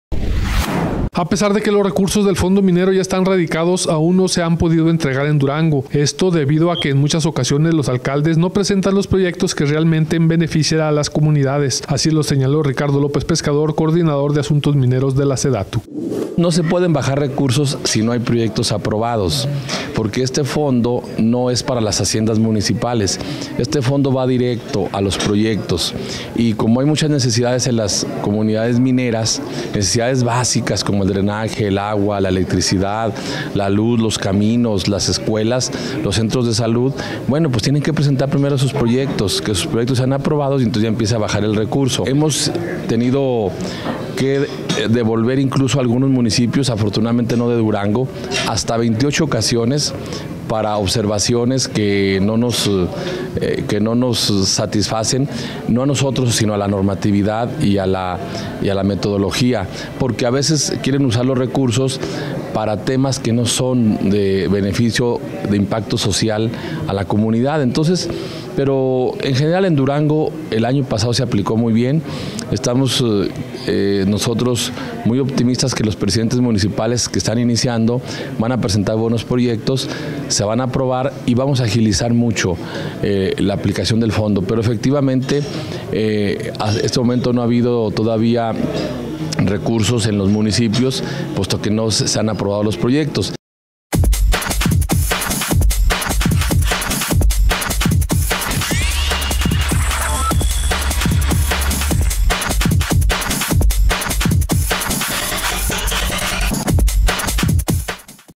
Alcaldes no presentan proyectos viables para sus comunidades
A pesar de que los recursos del fondo minero ya están radicados aún no se han podido entregar, esto debido a que en muchas ocasiones los alcaldes no presentan los proyectos que realmente beneficien a las comunidades, así lo señaló Ricardo López Pescador coordinador de asuntos mineros en la SEDATU